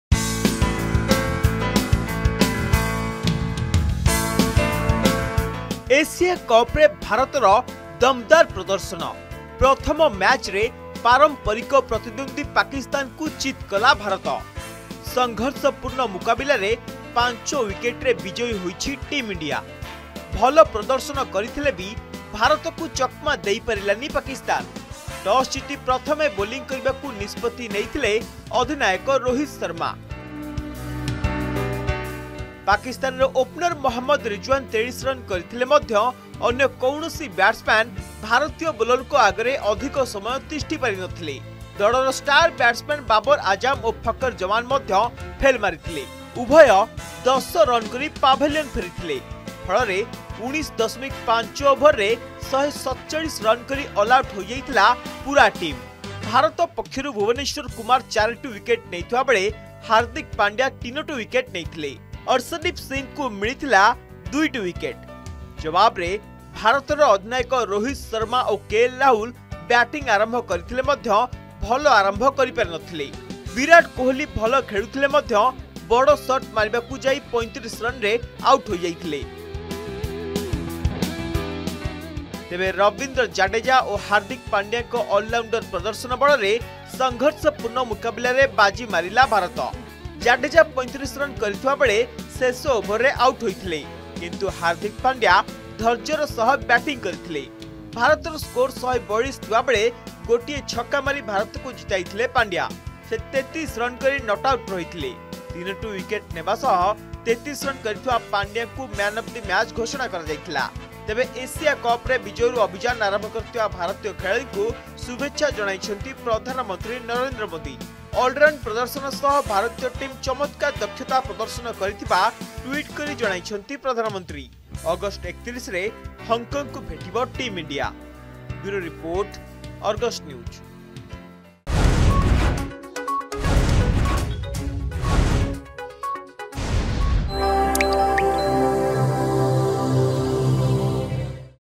IND vs PAK: India beats Pakistan by 5 wickets
India beats Pakistan by 5 wickets.